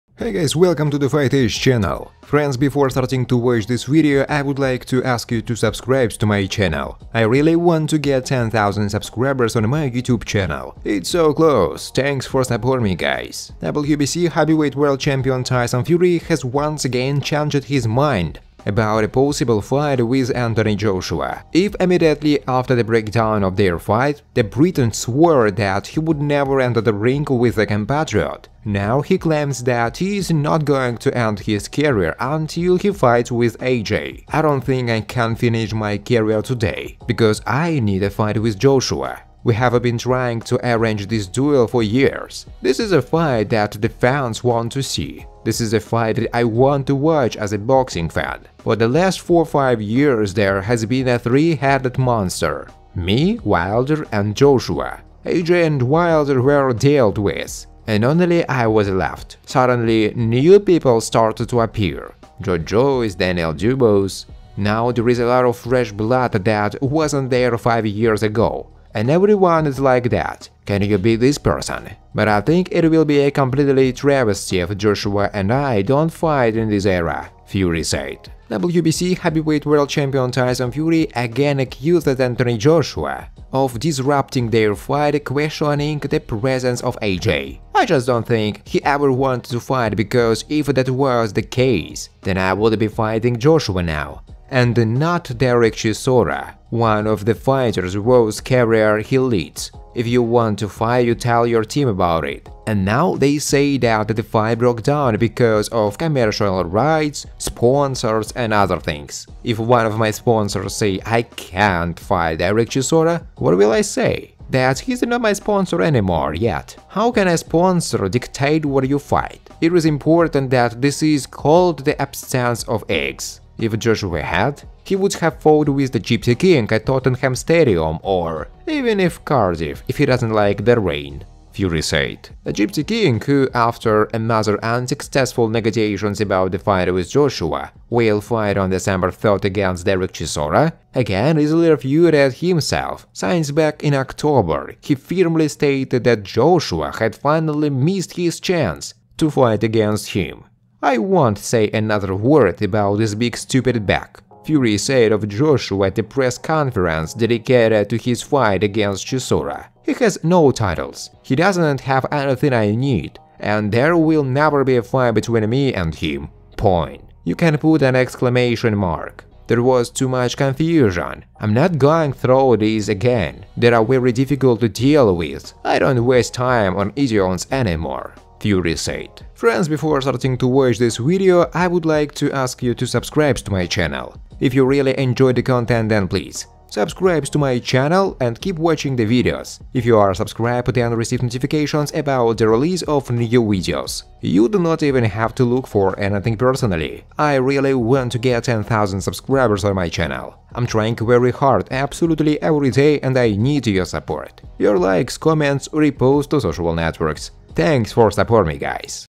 Anthony Joshua MADE ACCUSATIONS AGAINST Tyson Fury BECAUSE OF THE CANCELLATION OF THE FIGHT
WBC heavyweight world champion Tyson Fury has once again changed his mind about a possible fight with Anthony Joshua.

If immediately after the breakdown of their fight, the Briton swore that he would never enter the ring with a compatriot, now he claims that he is not going to end his career until he fights with AJ.

"I don't think I can finish my career today, because I need a fight with Joshua. We have been trying to arrange this duel for years. This is a fight that the fans want to see. This is a fight that I want to watch as a boxing fan.

For the last 4-5 years there has been a three-headed monster: me, Wilder and Joshua. Joshua and Wilder were dealt with, and only I was left. Suddenly new people started to appear: Joe Joyce, Daniel Dubois
Now there is a lot of fresh blood that wasn't there five years ago, and everyone is like that: "Can you beat this person?" But I think it will be a complete travesty if Joshua and I don't fight in this era," Fury said.

WBC heavyweight title holder Tyson Fury again accused Anthony Joshua of disrupting their fight, questioning the presence of A.J.

"I just don't think he ever wanted to fight, because if that was the case, then I would be fighting Joshua now, and not Dereck Chisora - one of the fighters whose career he leads. If you want to fight, you tell your team about it. And now they say that the fight broke down because of commercial rights, sponsors and other things.

It is important that this is called the absence of eggs. If Joshua had, he would have fought with the "Gypsy King" at Tottenham Stadium or even in Cardiff, if he doesn't like the rain," Fury said.

Fury, who, after another unsuccessful negotiations about a fight with Joshua, will fight on December 3 against Dereck Chisora, again easily refuted himself, since back in October he firmly stated that Joshua had finally missed his chance to fight against him.

"I won't say another word about this big stupid bag," Fury said of Joshua at a press conference dedicated to his fight against Chisora. — He has no titles. He doesn't have anything I need, and there will never be a fight between me and him. Point. You can put an exclamation mark. There was too much confusion. I'm not going through this again. They are very difficult to deal with. I don't waste time on idiots anymore."